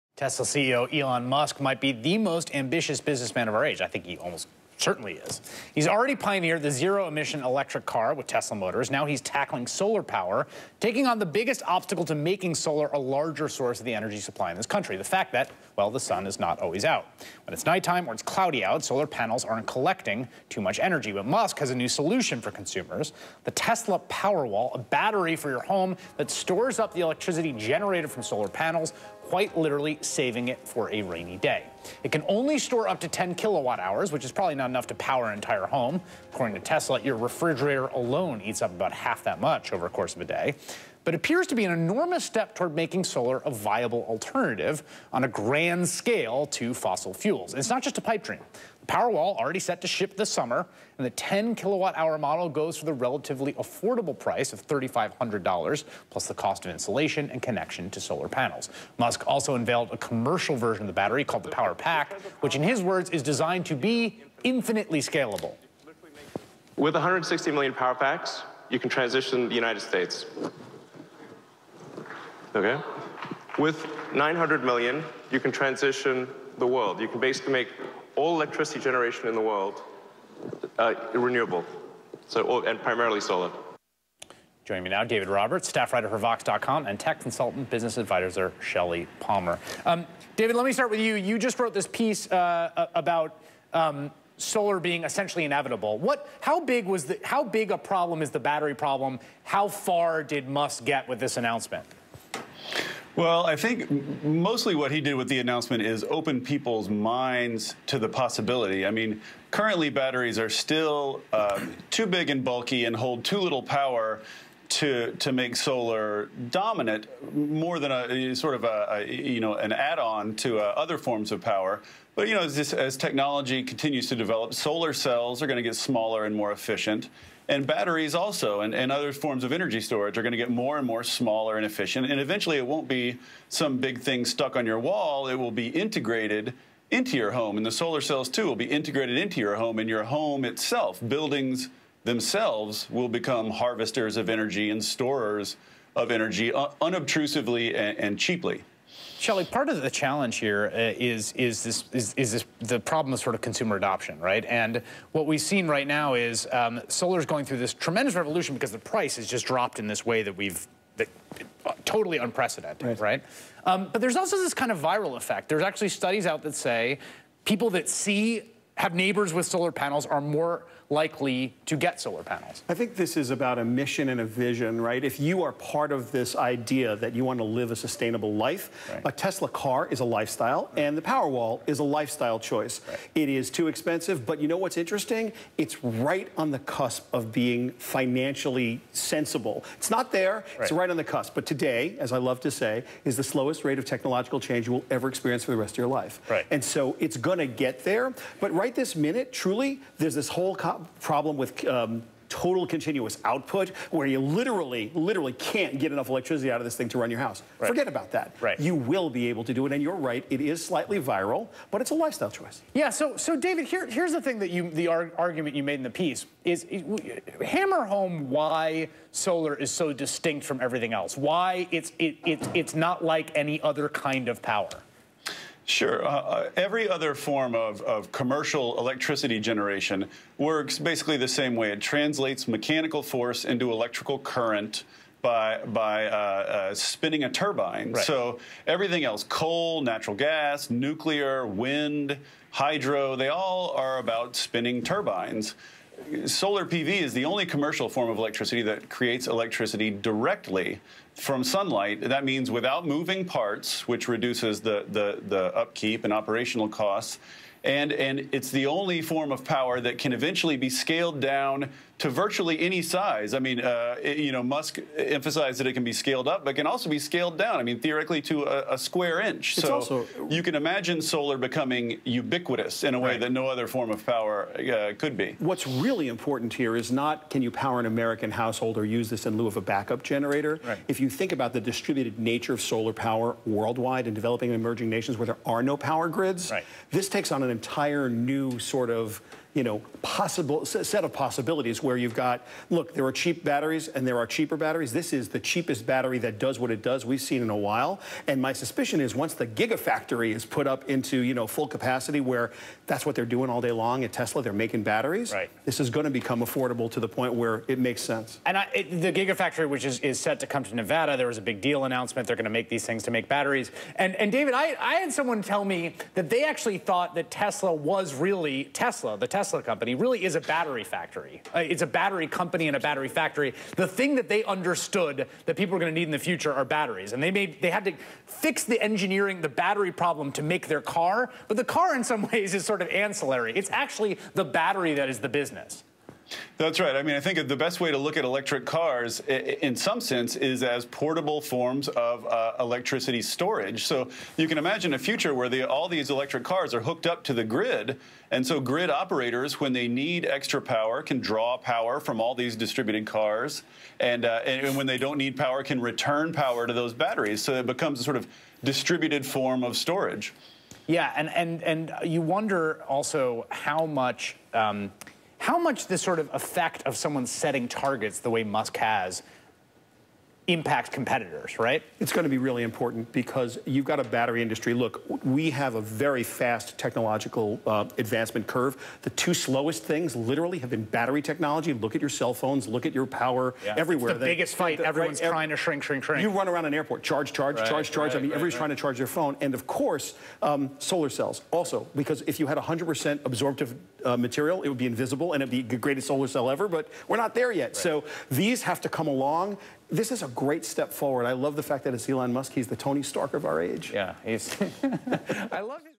Shelly Palmer Talks About Tesla's Power Wall With Chris Hayes on MSNBC
Shelly Palmer talks about Tesla's Power Wall Battery technology with Chris Hayes on MSNBC. Airdate: May 5, 2015